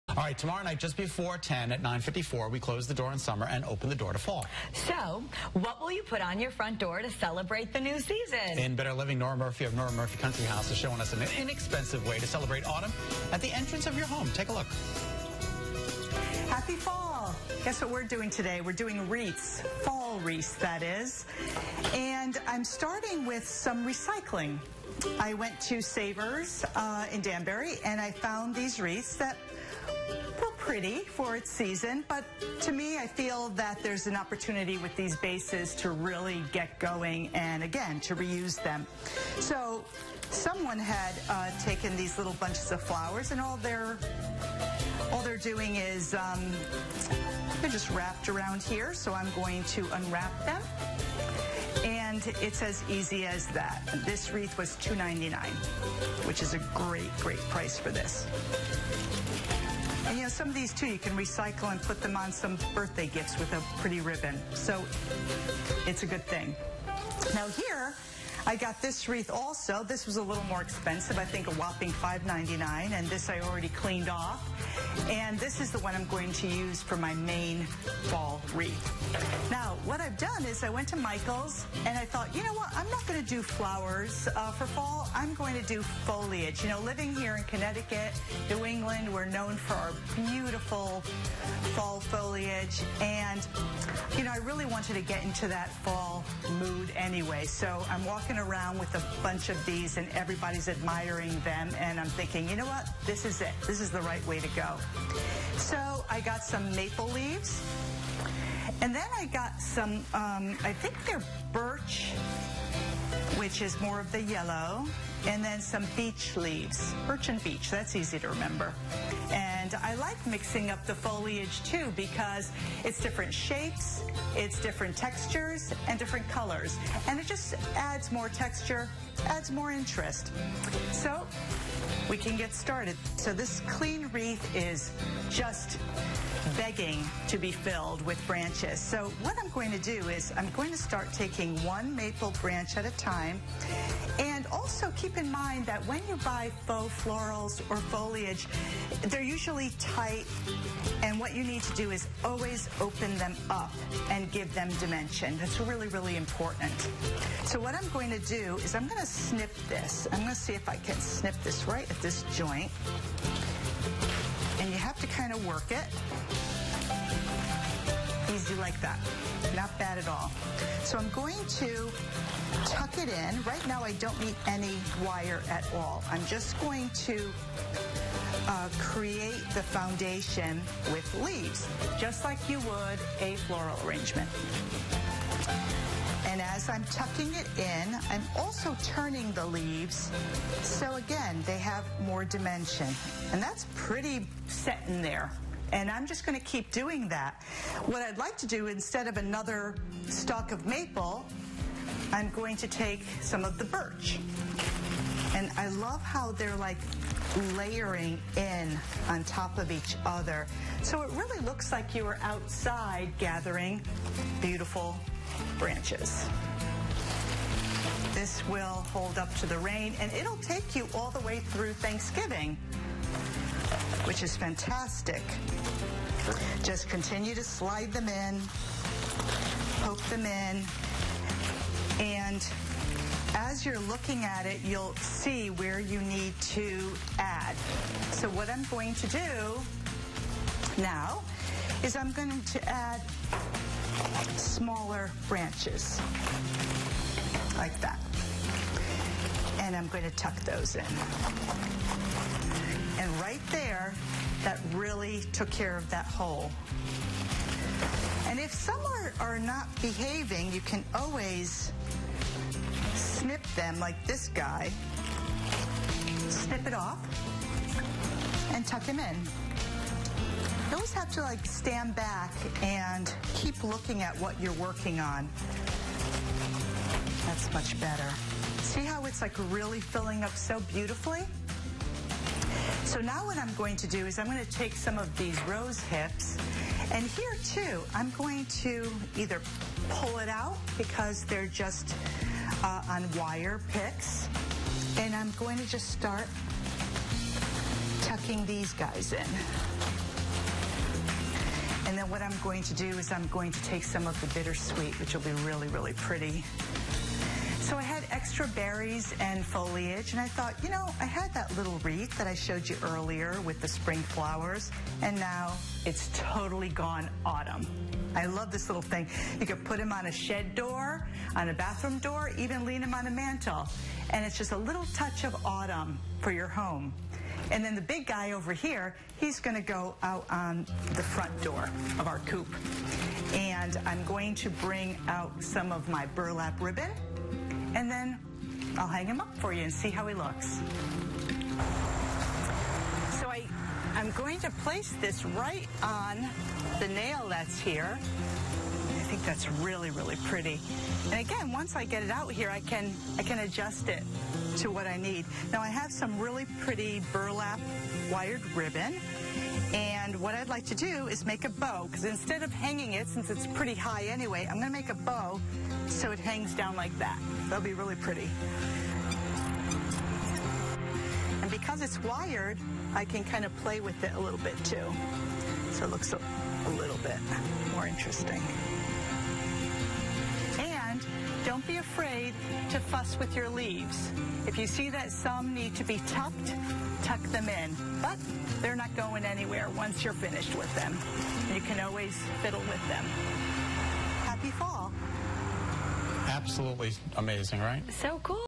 9 21 18 Making a Fall Wreath with Nora Murphy
If you're looking for a beautiful display for your front door, why not make it yourself.  Check out what Nora Murphy of Nora Murphy Country House created ... and it wasn't that expensive!